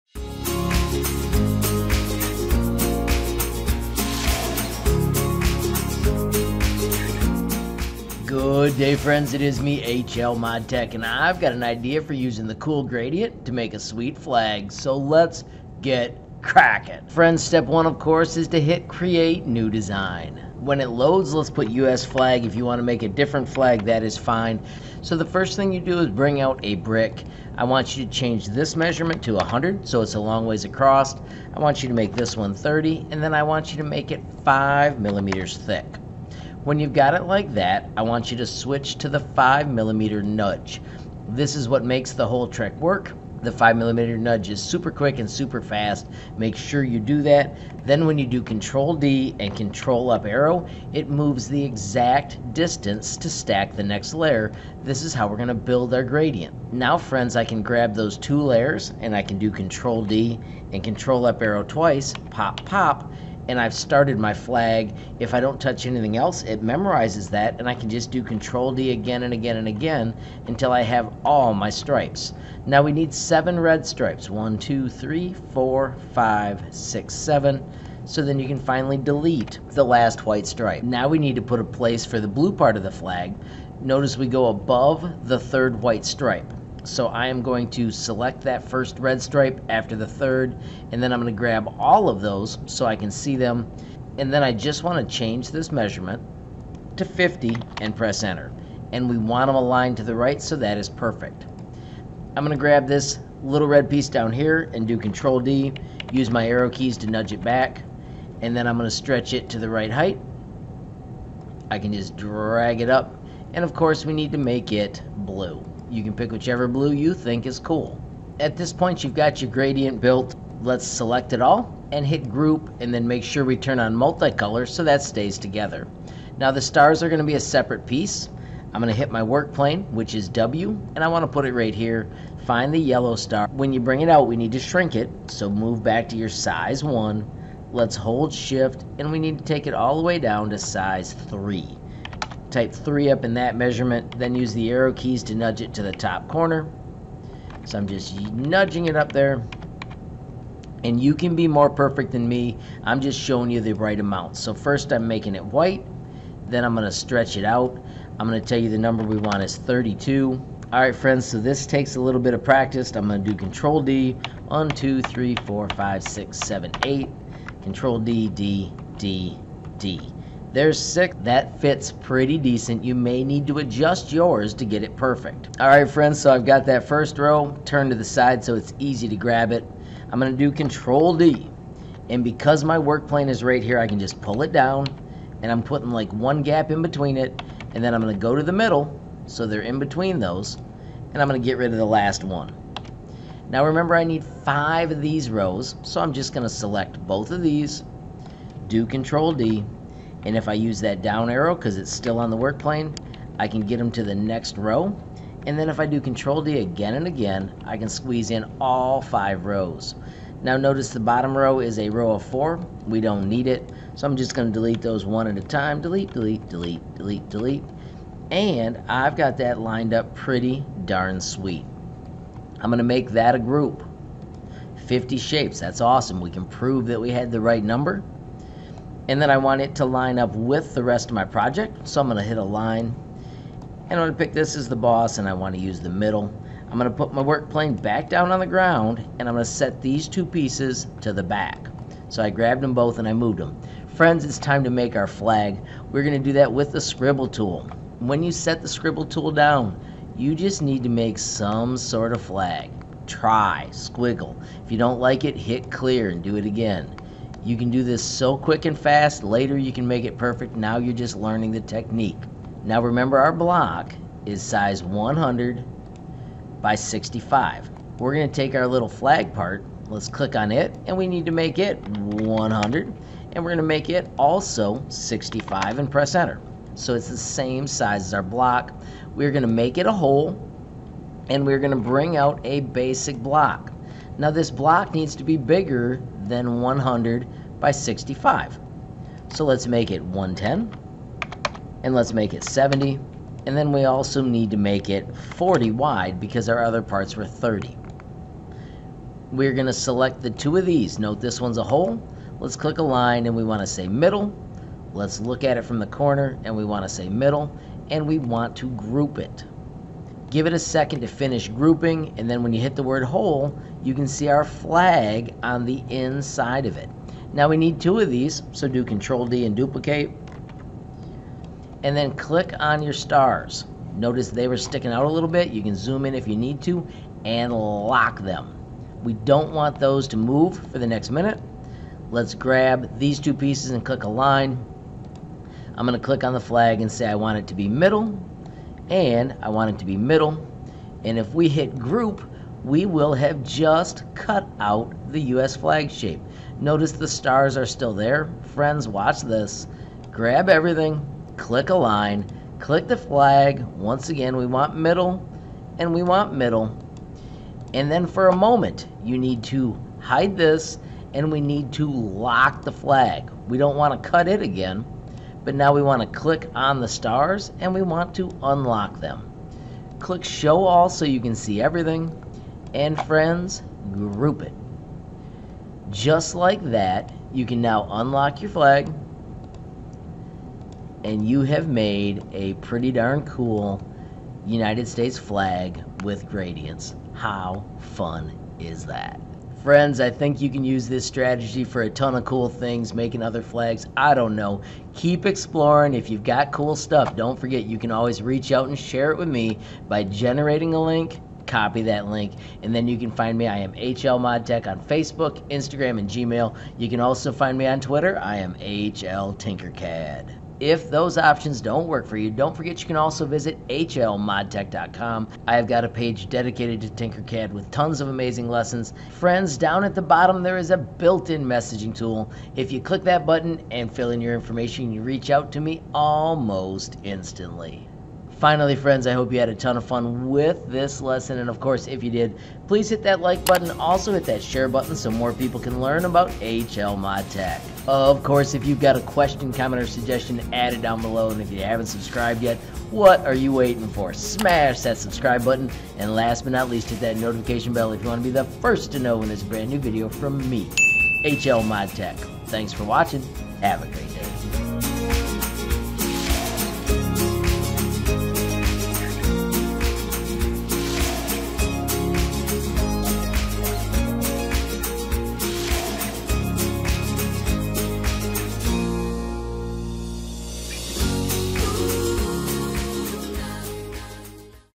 Create an Amazing Tinkercad Waving US Flag using Gradients
Have a glorious day and keep Tinkering! Create an Amazing Tinkercad Waving US Flag using Gradients. Using Gradients and objects as holes is a ton of fun. Today we even make our own Gradient!  Have fun and see where else you can use the amazing Gradient parts.